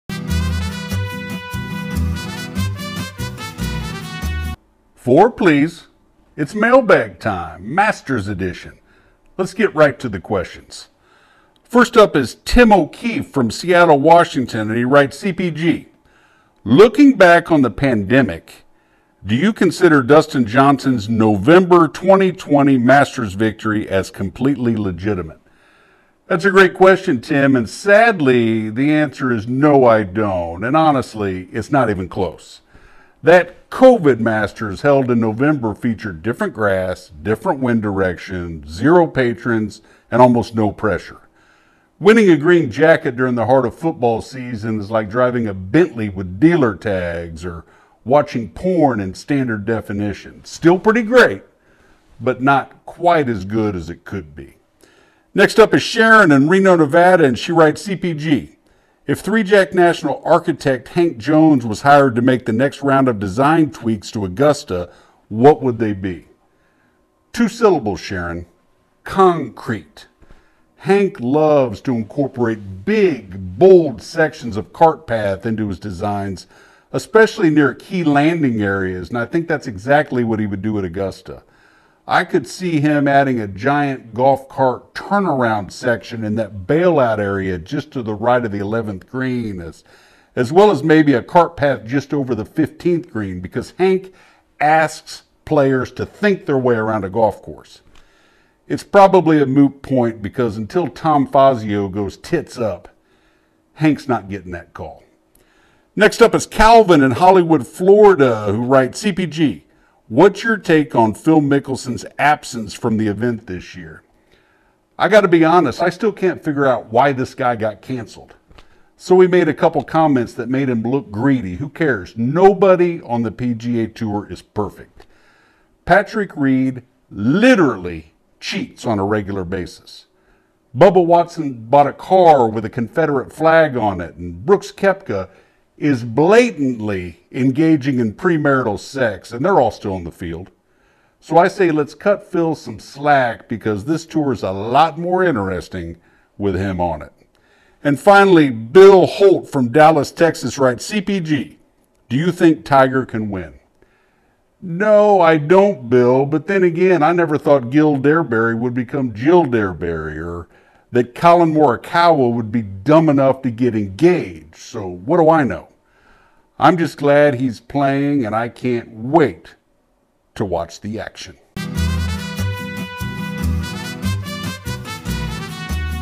Masters Mailbag
Watch as CPG answers questions from viewers about the much anticipated 2022 Masters.